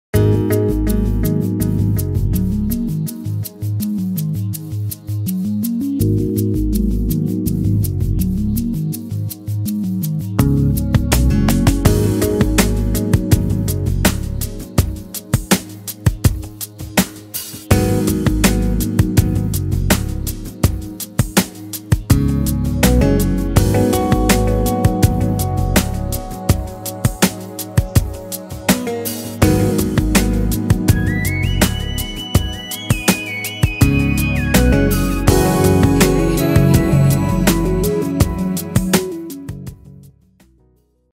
Ladies' Sekonda Crystalla Watch (4716)
Product demonstration Ladies' Sekonda Crystalla Watch (4716).